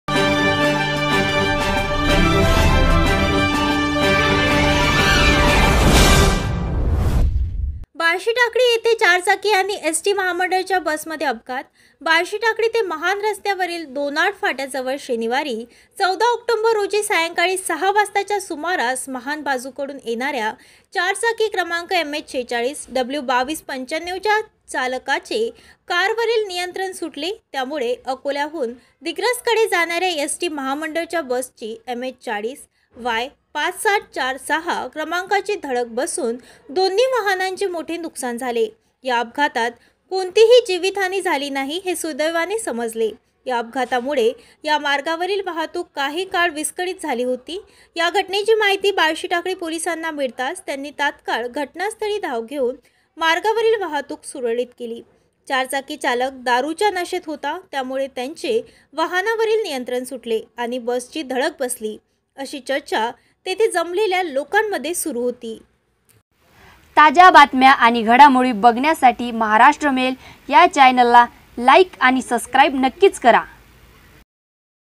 बार्शीटाकळी येथे चारचाकी आणि एसटी महामंडळ बसचा अपघात
*बार्शीटाकळी येथे चारचाकी आणि एसटी महामंडळाच्या बसमध्ये अपघात* बार्शीटाकळी ते महान रस्त्यावरील दोनाड फाट्याजवळ शनिवार, 14 ऑक्टोबर रोजी सायंकाळी 6 वाजेच्या सुमारास महान बाजूकडून येणाऱ्या चारचाकी क्रमांक mh46 w2295 च्या चालकाचे कारवरील नियंत्रण सुटले. त्यामुळे अकोल्याहून दिग्रसकडे जाणाऱ्या एसटी महामंडळाच्या बसची mh40 y5746 क्रमांकाची धडक बसून दोन्ही वाहनांचे मोठे नुकसान झाले.या अपघातात कोणतीही जीवितहानी झाली नाही हे सुदैवाने समजते. या अपघातामुळे या मार्गावरील वाहतूक काही काळ विस्कळीत झाली होती. या घटनेची माहिती बार्शिटाकळी पोलिसांना मिळताच त्यांनी तत्काळ घटनास्थळी धाव घेऊन मार्गावरील वाहतूक सुरळीत केली. चारचाकी चालक दारूच्या नशेत होता. त्यामुळे त्यांचे वाहनावरील नियंत्रण सुटले आणि बसची धडक बसली. अशी चर्चा तिथे जमलेल्या लोकांमध्ये सुरू होती.